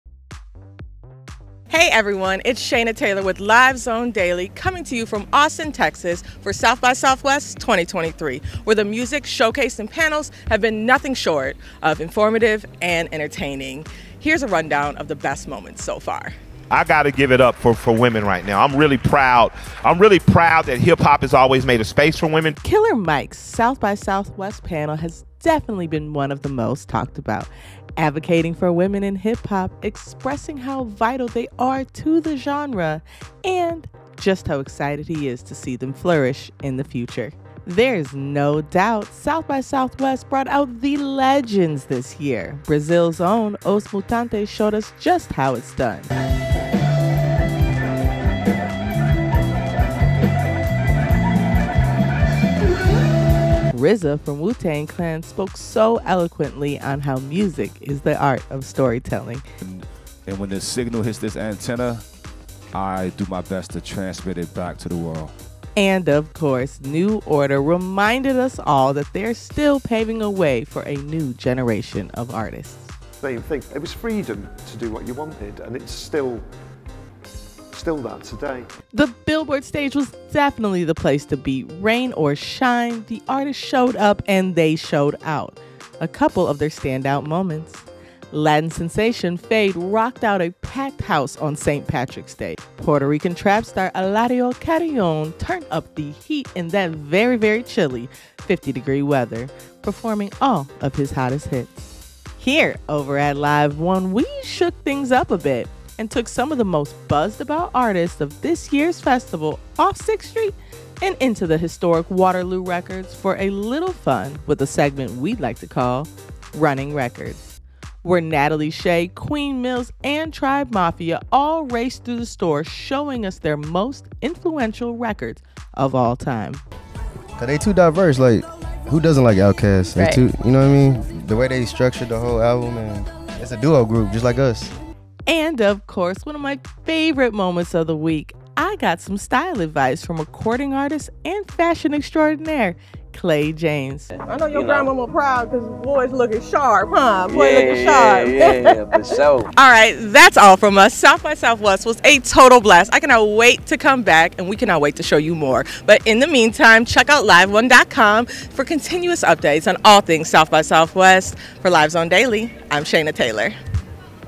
Shaynna SXSW Wrap Up
Covering SXSW2023 was a total blast!
I was on location as a Producer & Host for LiveOne as we tracked down the most buzzed about up & coming artists & covered some of the biggest events & showcases.
Killer Mike's Panel was excellent as he advocated for women in hop-hop and continues to be a staple in the hip-hop community.
RZA from Wu-Tang Clan always speaks so eloquently & gives motivational advice during his panel.
The Billboard stage brought out so many performances, my top were Feid & Eladio Carrion! These Latin sensations were on fire.
And at LiveOne we had some amazing interviews, like Clay James, Bella, and Tribe Mafia. We even took some of them into a historic record store to play a fun game to get to know these artists a bit better.
Get ready as I grace your screens just a bit more.

Credit
LIVEONE
LIVE ZONE DAILY